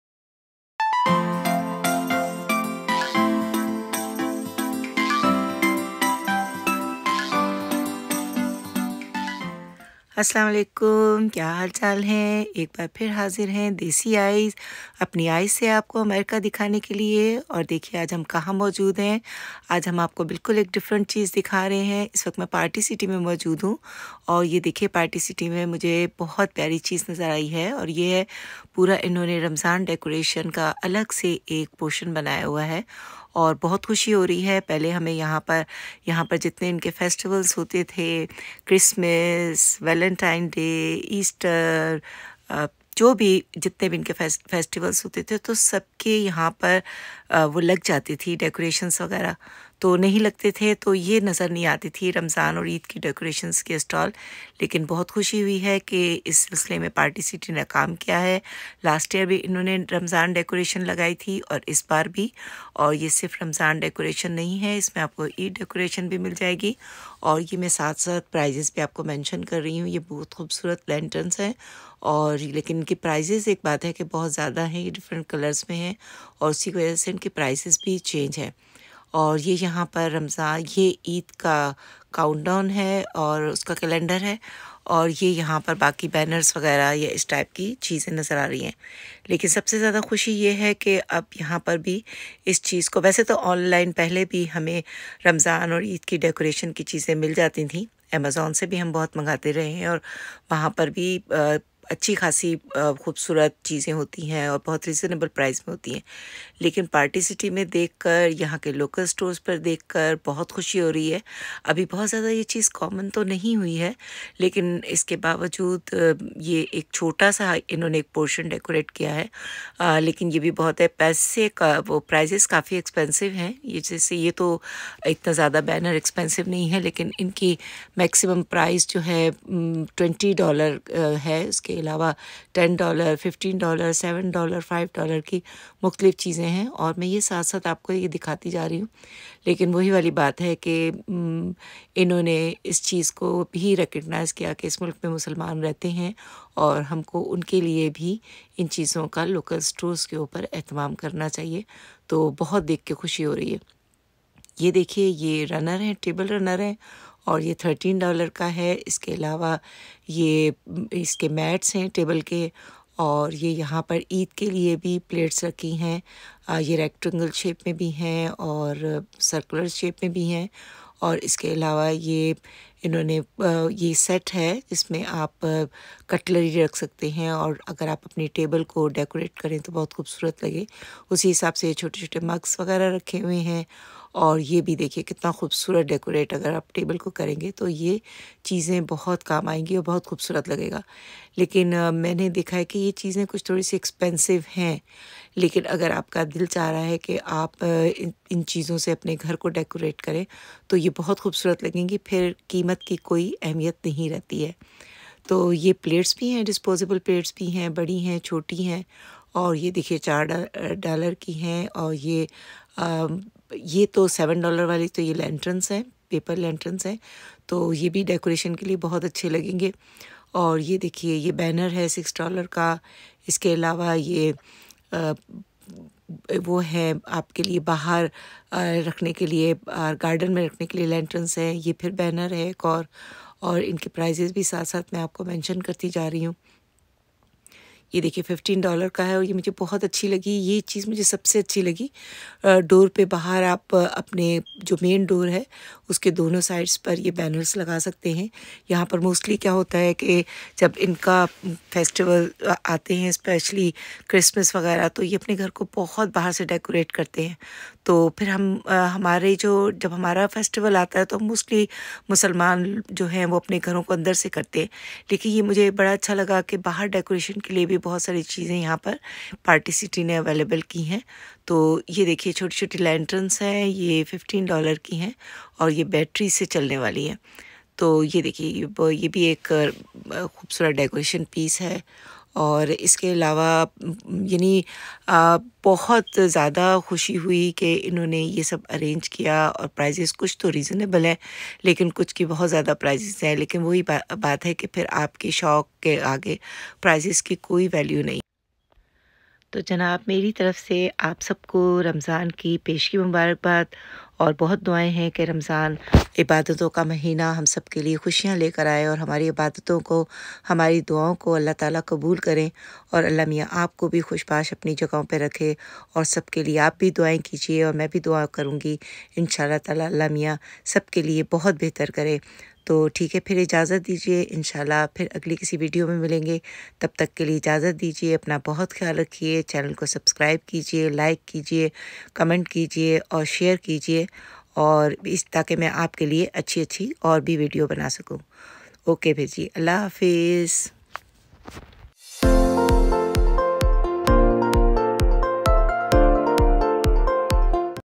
Ramadan Decorations supplies at Party City||Ramadan Kareem
Ramadan Decorations at Party City,Ramadan is the 9th holy month of Hijri calendar.Muslims offer especial prayers and observe fast for whole month,they also decorate their homes according to this holy month.In video I visited to party city and amazed to see the especial counter for Ramadan Decorations 😀come with me and have a look,thanks